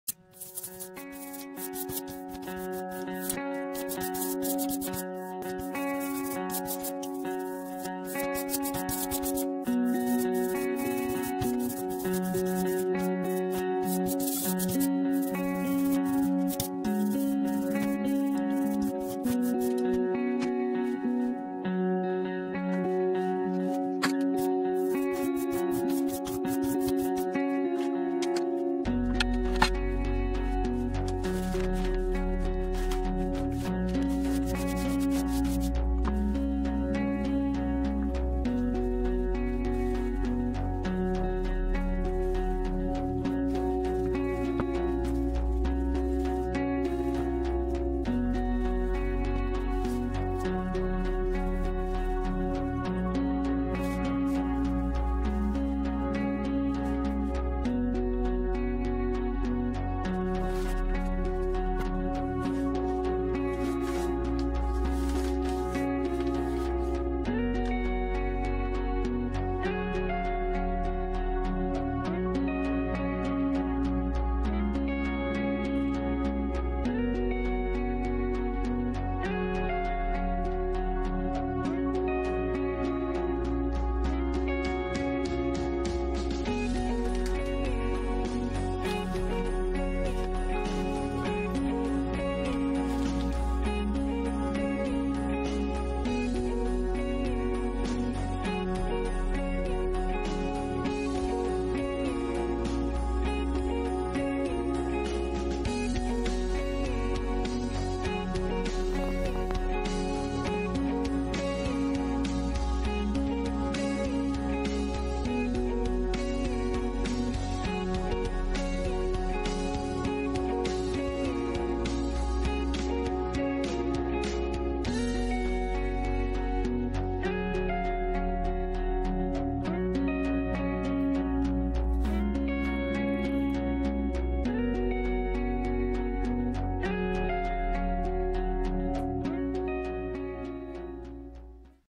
Print and reveal - linocut 'Old woman who swallowed a fly'
Quick video of my latest linoprint. Old woman who swallowed a fly.
This print is 44 x 65 cm
This is my first test prints and some minor adjustments are needed.

and you can sign up to my newsletter for more information about me and my art courses. https:/mailchi.mp/6133becb5cc8/artwithtricia